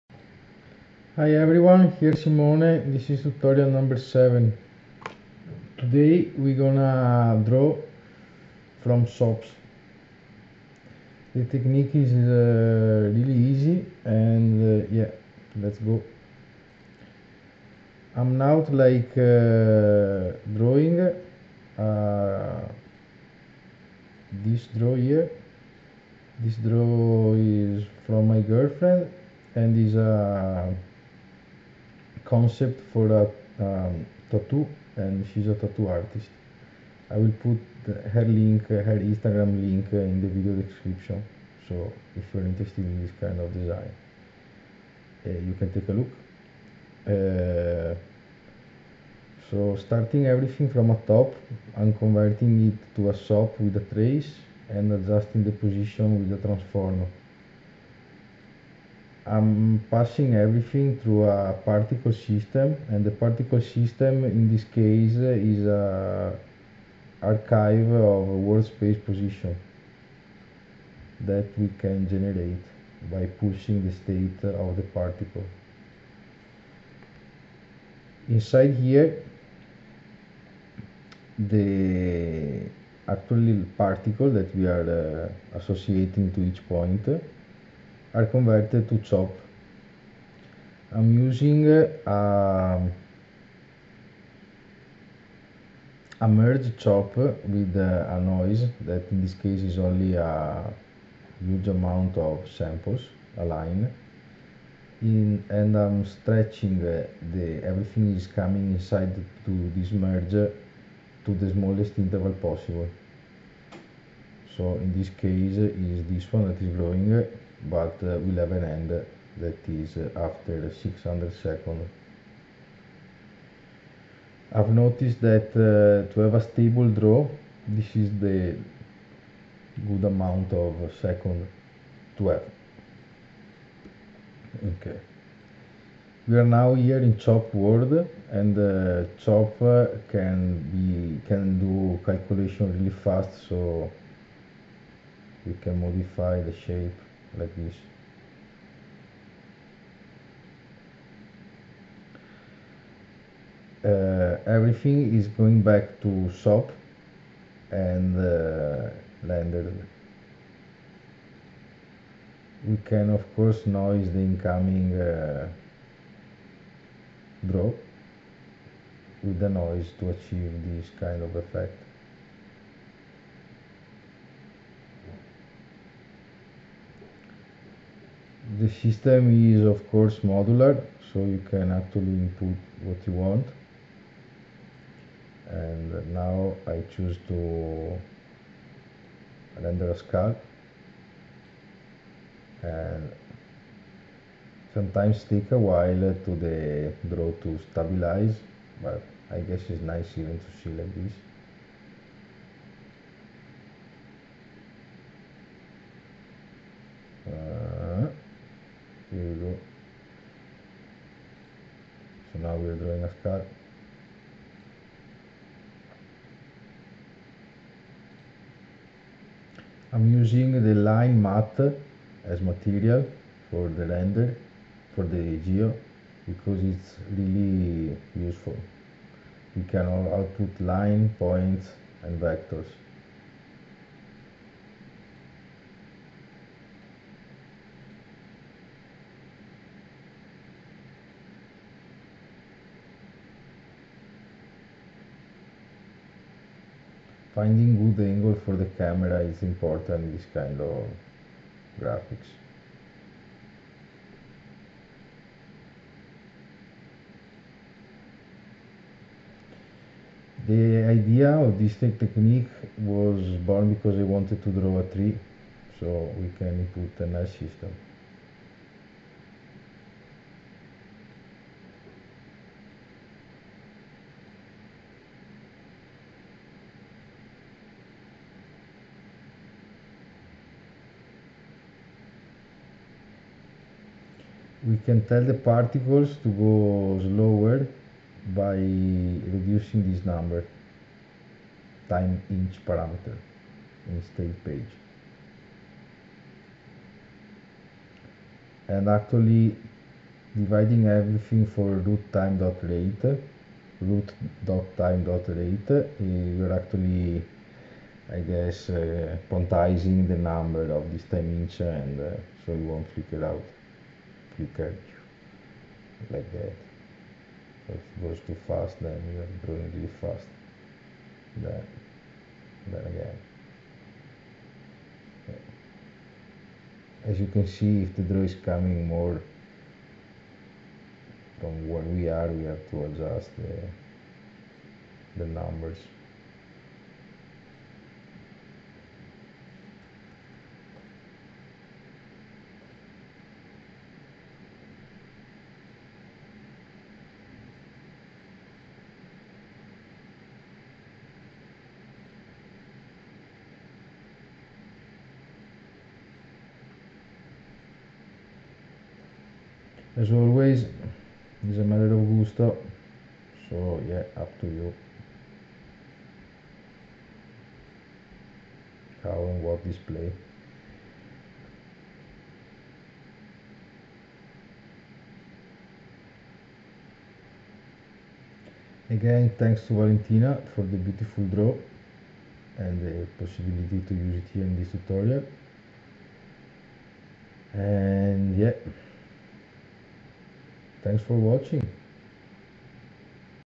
07 Shape Drawing with particles
In this tutorial we will take a look to a simple technique to draw from Sops operators.

Main focus on : particle sop as storage of world space position data.